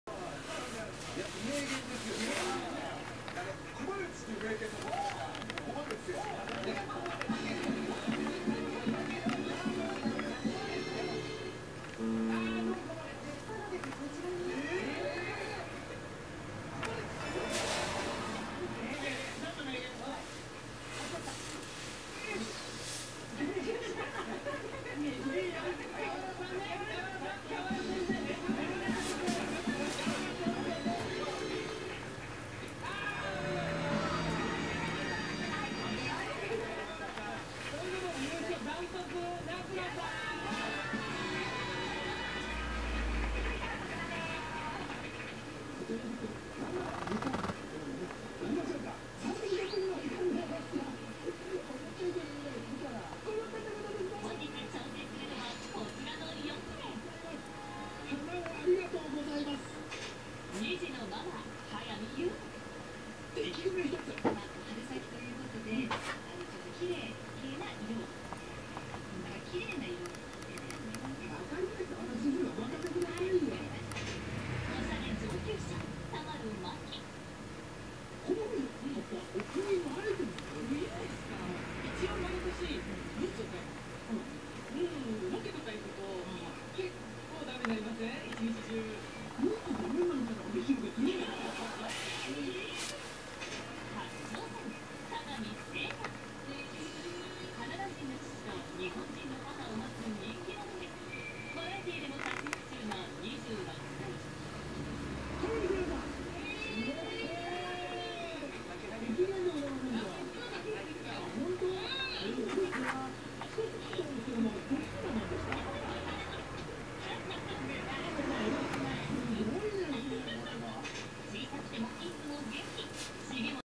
オタマジャクシと思っていたのが？徳島県から発信しています。
カエルの子オタマジャクシと思って持ち帰った卵！
動きだしたので撮影してみたら 今まで見てきた形がオタマジャクシとは全然違う形！
はてな？なんなんでしょう？
こんな寒い時期に孵化するかな　雪が降ってます。2014年2月7日寒いです。